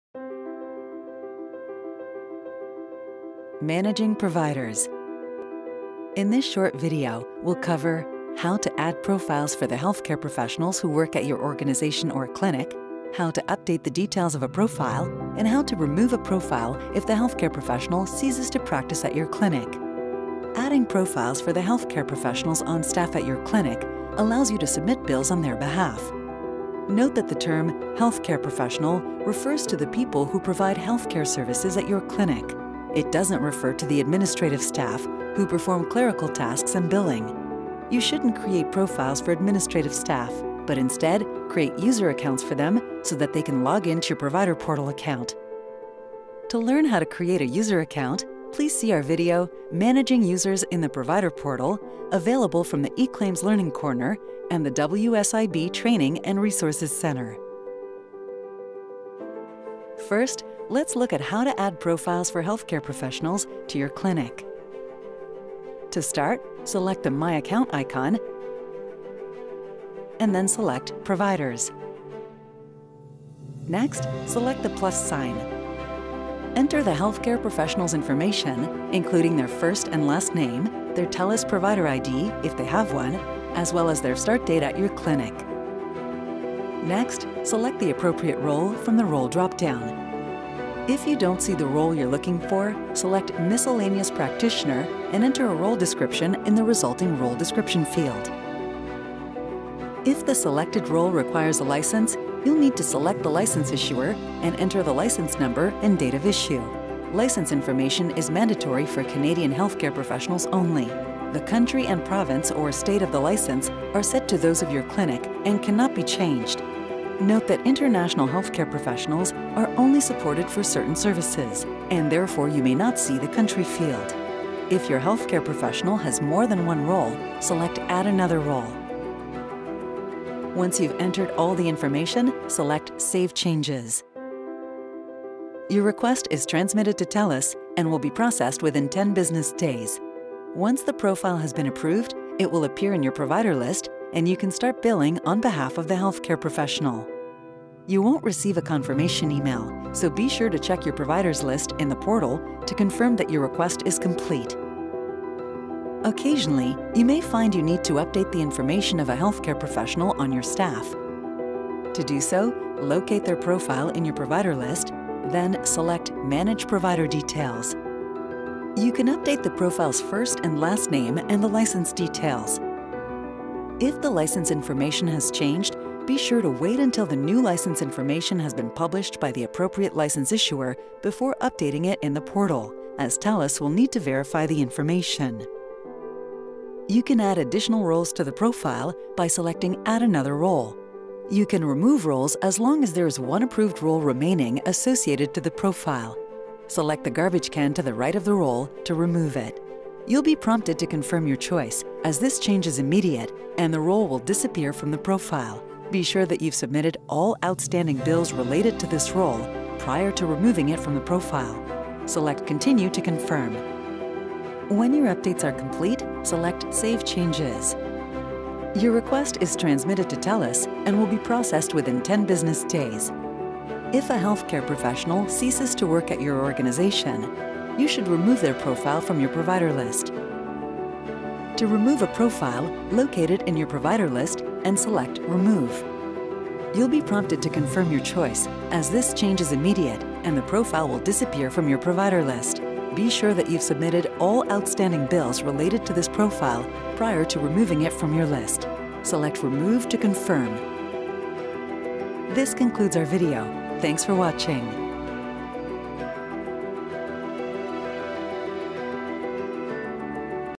Managing Providers | TELUS Health eClaims
This video explains how to add, update and remove provider profiles from your clinic's provider portal account.